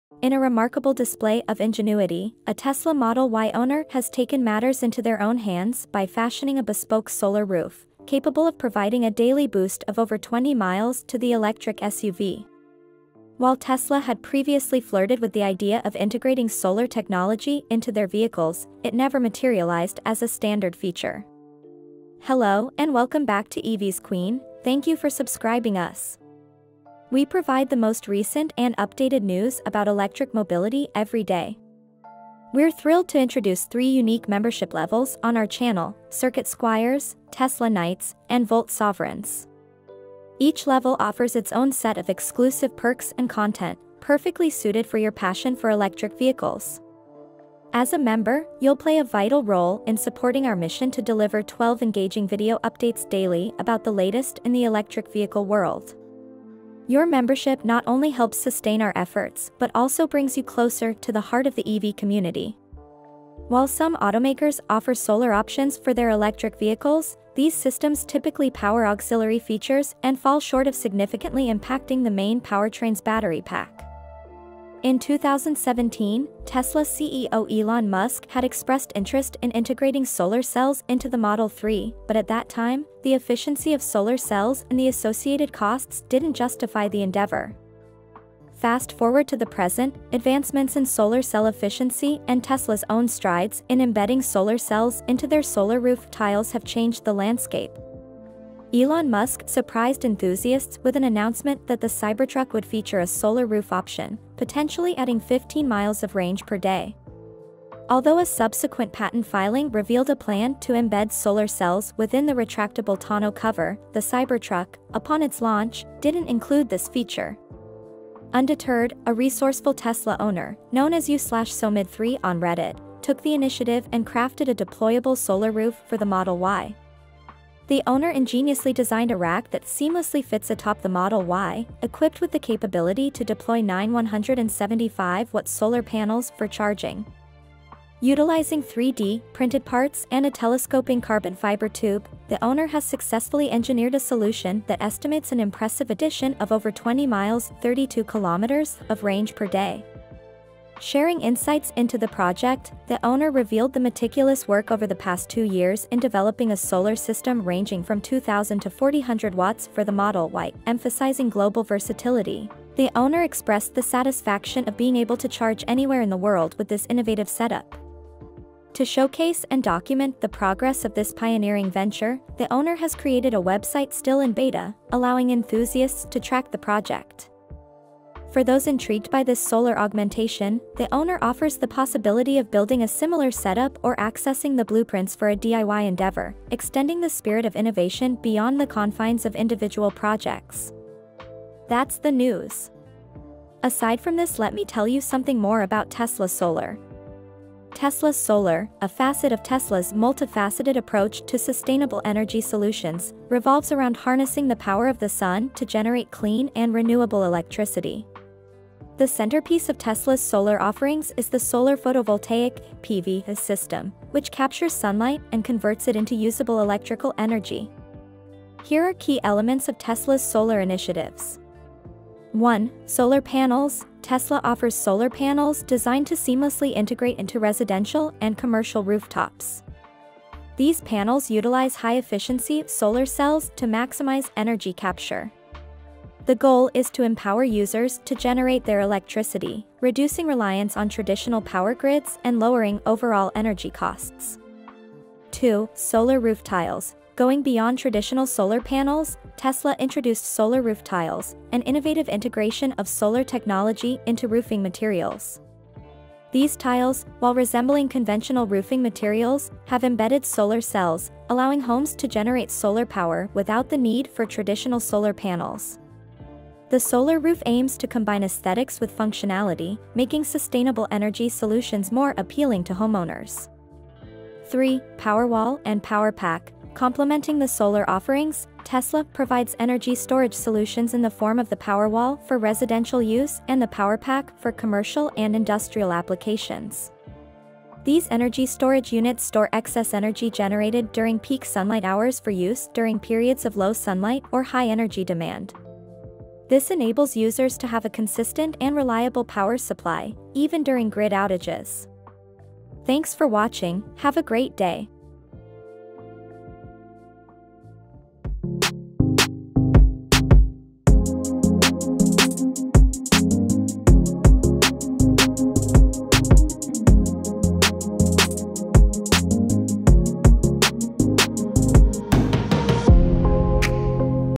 Tesla Enthusiast Constructs a Deployable Solar Roof for the Model Y
Exploring the New Tesla Model Y RWD and Skoda Enyaq Coupé and the Hyundai Ioniq 5